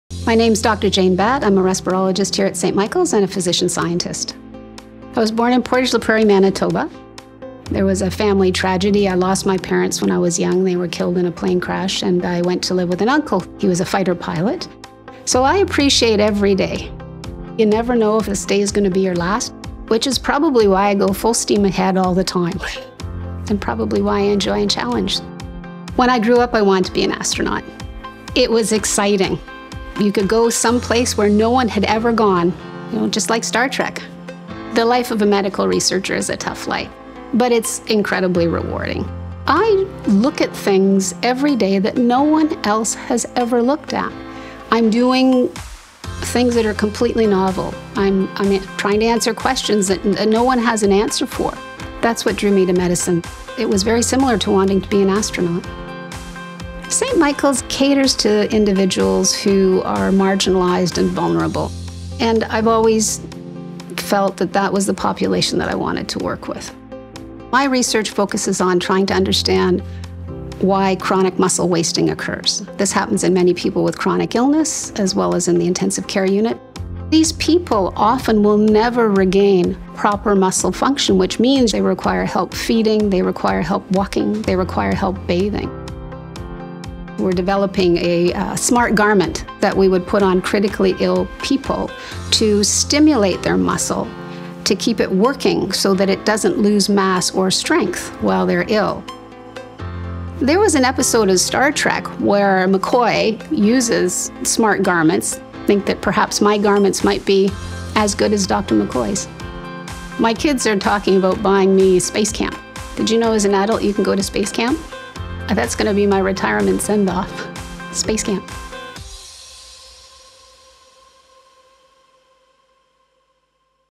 ANGELS DEN 2020: Smart Garments: AI Therapy For Your Muscles
Dr. Jane Batt, Scientist, Keenan Research Centre for Biomedical Science, and Respirologist,
St. Michael's Hospital

THE CHALLENGE: People who are critically ill often see their muscles waste, for good. While the condition might just be prevented with neuromuscular electrical stimulation (NMES), the current technology is practically and economically unfeasible.

THE SOLUTION: Invent “smart textile” garments, like leg stockings and arm sleeves, that automate NMES stimulation through the power of AI.